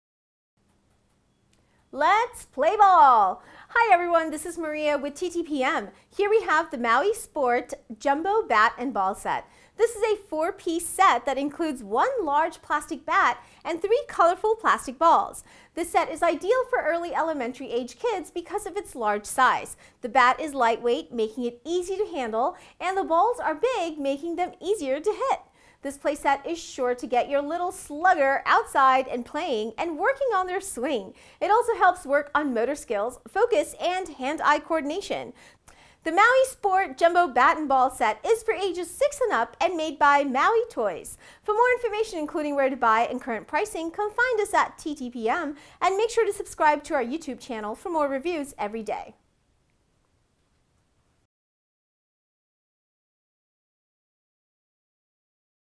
Maui Sport Jumbo Bat and Ball from Maui Toys
Batter up! Maui Sport Jumbo Ball and Bat set. Comes with large, colorful plastic bat and 3 plastic balls. The large ball-and-bat set makes it easy for younger kids to hit the ball. Ages 6 and up. Maui Toys. For full review and shopping info

Product Info: Maui Sport Jumbo Ball and Bat set comes with a 21.5-inch fat, colorful plastic bat and three 2.5-inch plastic balls. The large ball-and-bat set will make it easy for younger kids to hit the ball. This staple of summer fun is for ages 6 and up from Maui.